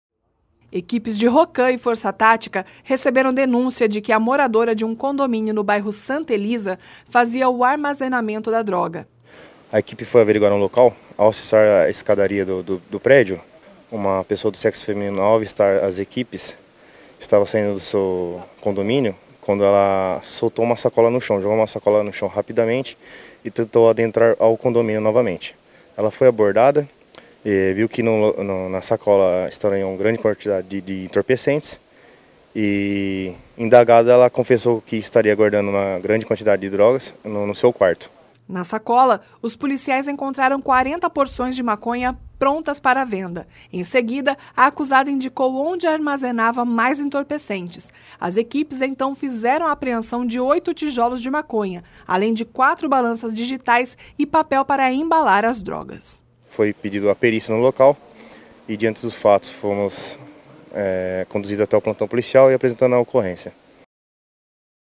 Polícia Militar apreende drogas no Santa Elisa
As equipes tiraram de circulação  8 tijolos de maconha e uma mulher foi presa.